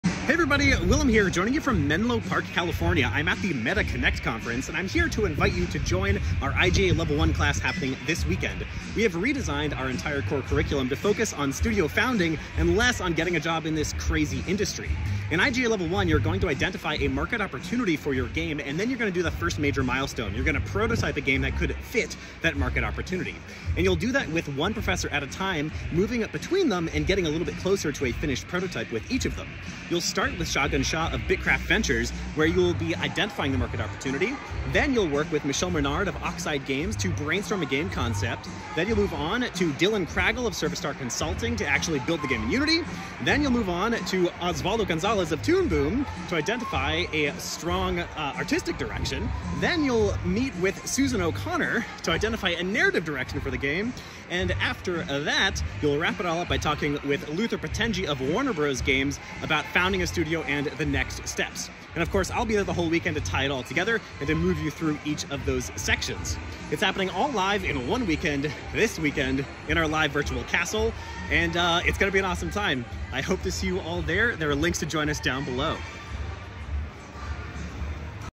Make your first viable unity game in 1 weekend 🔥
Join us for a brand new iteration of IGA Level 1! Take your first steps towards founding a video game studio and build a viable prototype in Unity under the guidance of 7 expert professors, THIS WEEKEND!

In IGA Level 1: Embark you will build one of your first games inside the Unity Game Engine, this time a 3D Galaga clone. But that's not all! For the first time we've redesigned Level 1 to focus on Indie Studio Founding, and specifically the first major milestone of game development: prototypes.

Over 8 live class hours you'll go from knowing nothing to having a complete game prototype of your own promising 3D Galaga clone designed and built entirely by you. You'll move through class time with 7 expert professors, each improving the quality of your prototype  with you.

1️⃣ Start with Shagun Shah of BITKRAFT Ventures to identify a market opportunity
2️⃣ Work with Michelle Menard of Oxide Games to brainstorm a game to fit this opportunity
3️⃣ Get help from Dylan Cragle of Servistar Consulting to build the prototype in Unity
4️⃣ Collab with Osvaldo Gonzalez of Toon Boom to identify a Visual Direction for your game
5️⃣ Learn from Susan O'Connor of The Narrative Department to pick a Narrative Direction
6️⃣ Talk with Luther Patenge of Warner Bros Games on Indie Studio Founding
7️⃣ And finish with yours truly to wrap it up, talk next steps, and graduate!

All that in just one weekend, live in our VIRTUAL CASTLE!

Oh and NO PRIOR DEVELOPMENT EXPERIENCE REQUIRED! This class is meant for all who dream of making  games. ✨

SEE YOU THEN EVERYONE! 🚀

What game would you be excited to make in our next Level 1?